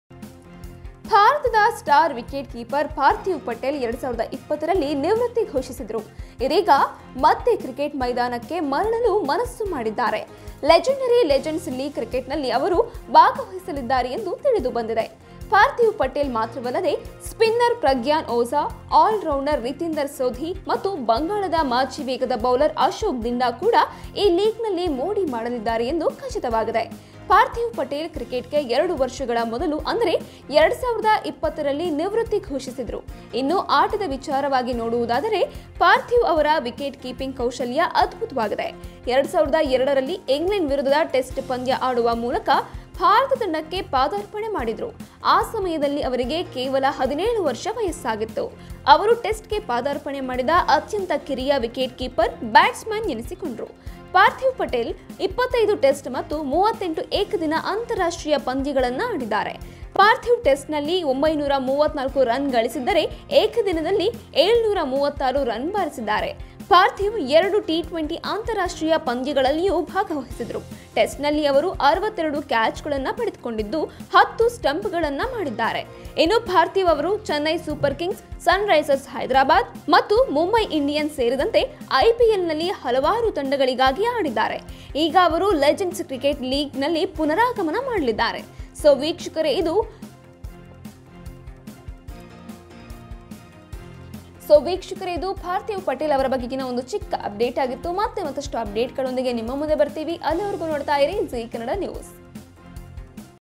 ಮತ್ತೆ ಕ್ರಿಕೆಟ್ ಮೈದಾನಕ್ಕೆ ಮರಳಲು ಮನಸ್ಸು ಮಾಡಿದ ಪಾರ್ಥಿವ್ ಪಟೇಲ್. || ZEE KANNADA NEWS
ಭಾರತದ ಸ್ಟಾರ್ ವಿಕೆಟ್‌ ಕೀಪರ್ ಪಾರ್ಥಿವ್ ಪಟೇಲ್ 2020ರಲ್ಲಿ ನಿವೃತ್ತಿ ಘೋಷಿಸಿದ್ದರು. ಇದೀಗ ಮತ್ತೆ ಕ್ರಿಕೆಟ್ ಮೈದಾನಕ್ಕೆ ಮರಳಲು ಮನಸ್ಸು ಮಾಡಿದ್ದಾರೆ.. ಆ ಕುರಿತ ಡೀಟೈಲ್ಸ್‌ ಇಲ್ಲಿದೆ.